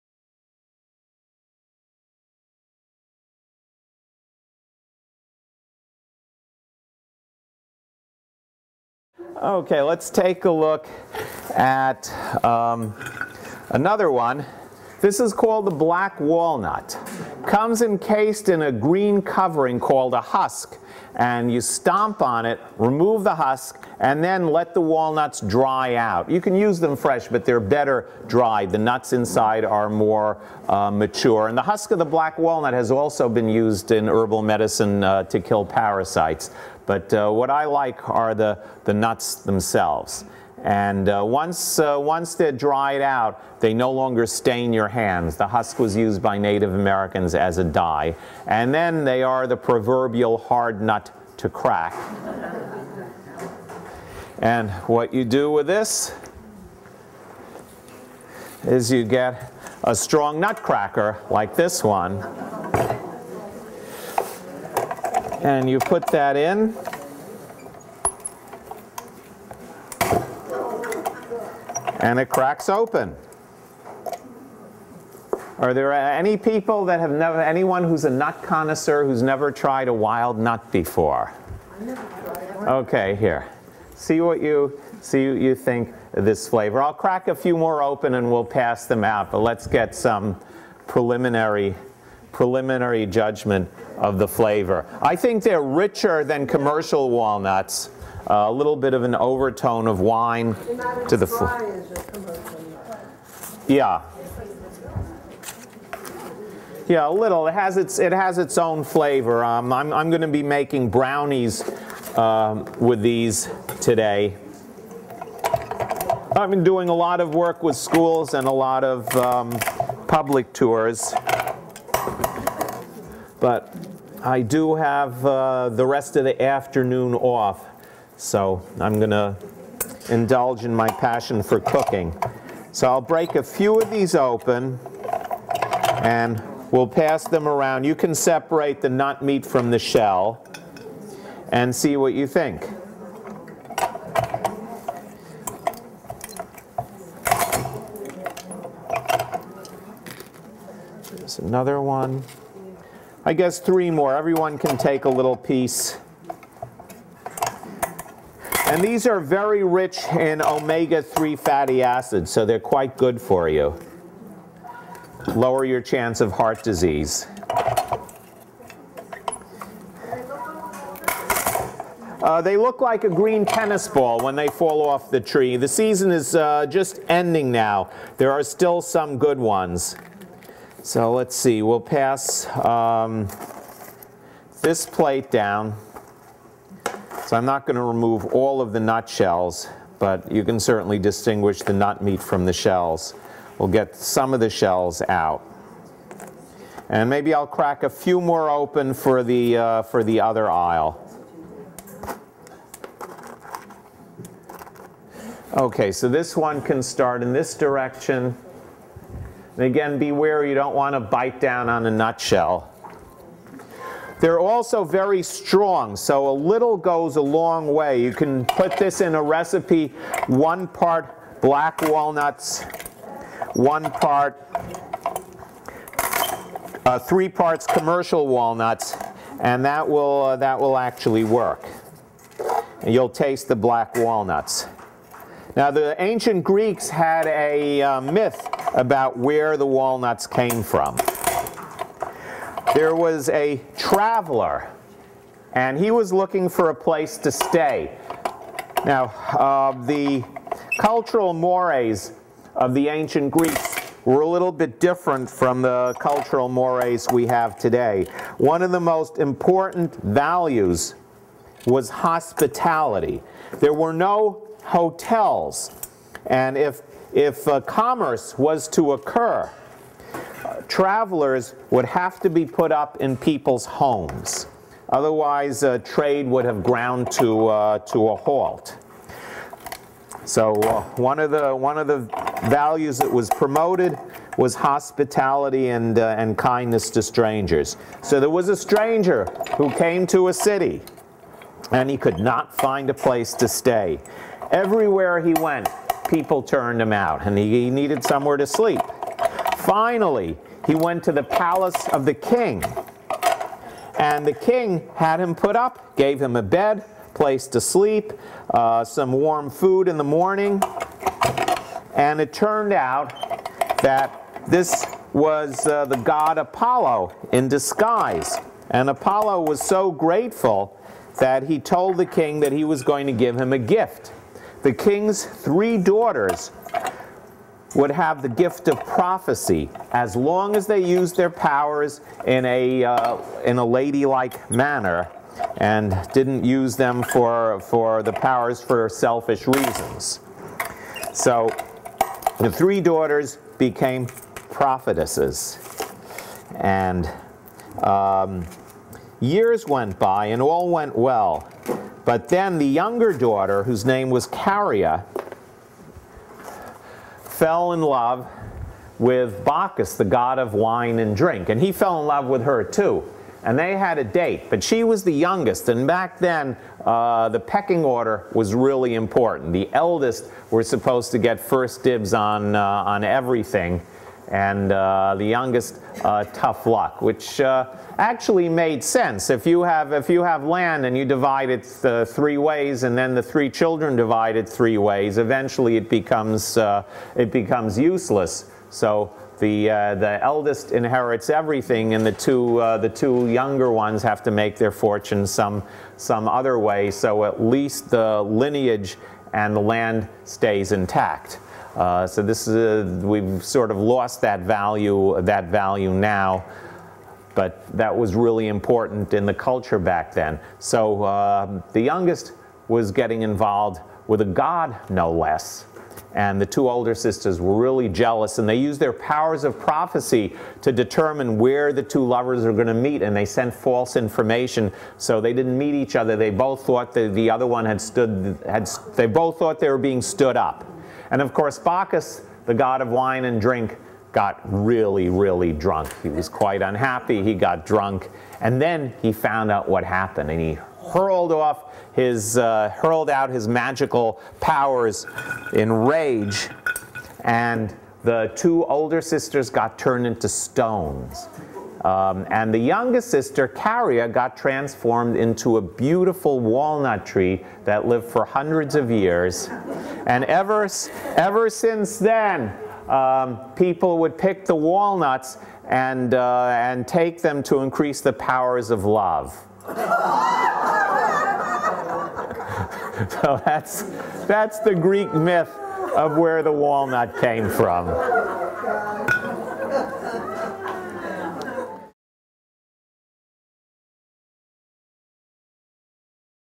0004. Black Walnut.m4v
Learn about this delicious, common wild nut, part 4 of my talk at the Pleasantville (NY) Garden Club